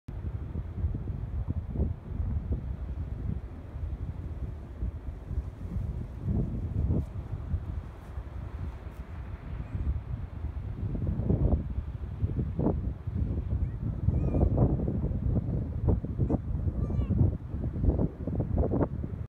Spurn Point
A panoramic video of Spurn Head, a coastal spit to the the south of the Holderness Coast.